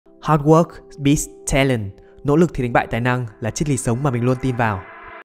NỖ LỰC có thể đánh bại TÀI NĂNG? | SHINPHAMM
Lúc nào mình cũng muốn ở xung quanh những người khiêm tốn + cầu thị + nỗ lực + kiên trì để nhắc nhở bản thân phải cố gắng hơn nữa!!! Mình không muốn thoả mãn bản thân một cách dễ dàng!!!
Nhất là nhìn thấy những người thành công rồi mà vẫn cố gắng…luôn làm mình cảm thấy rất phấn khích 🤘🏻